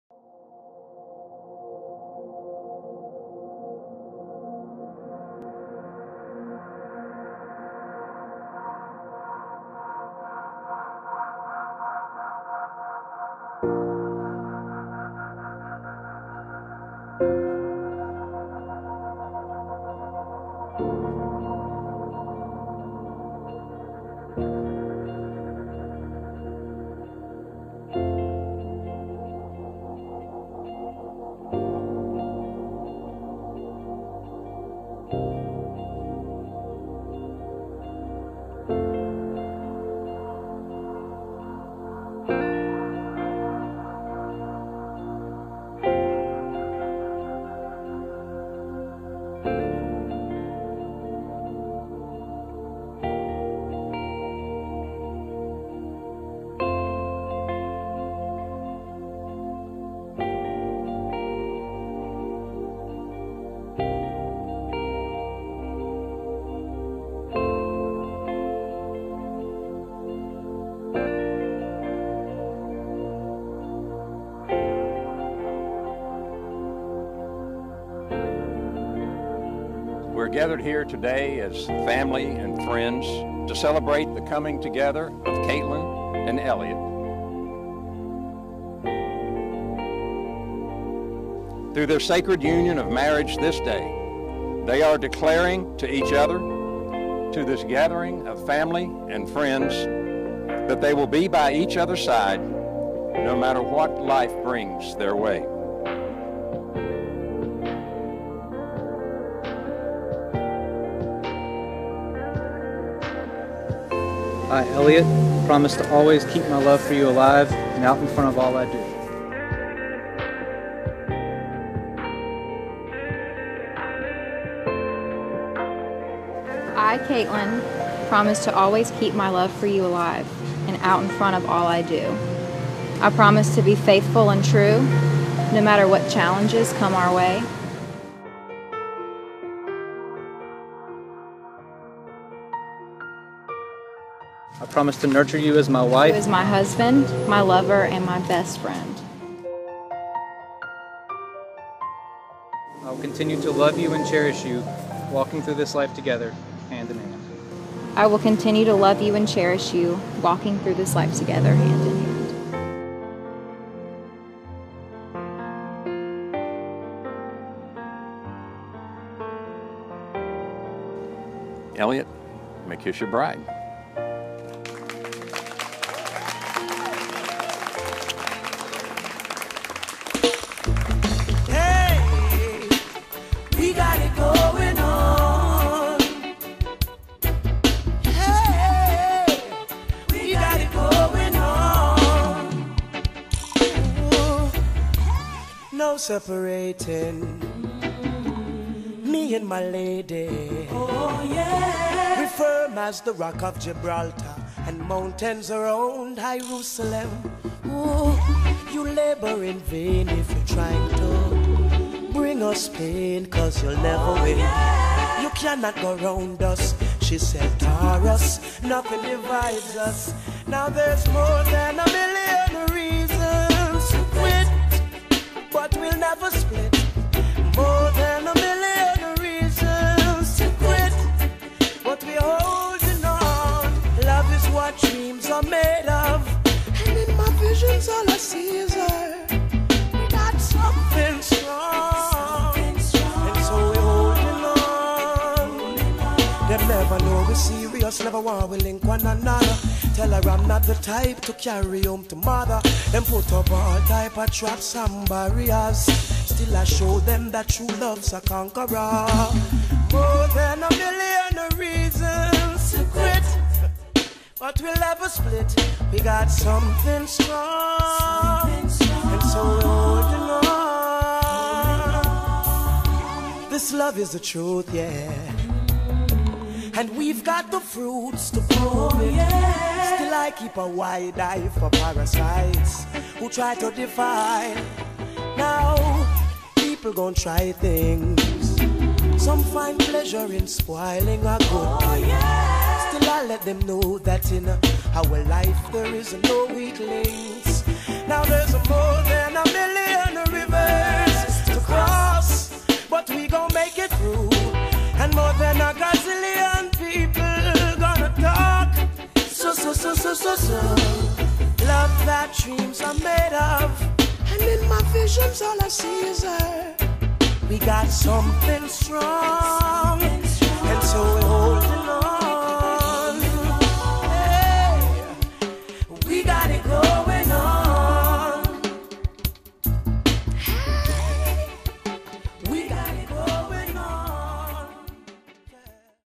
Millinor Wedding: September 15, 2017
Elliott and Katelyn are married on September 15, 2017 at Cafe Rule in Hickory, NC.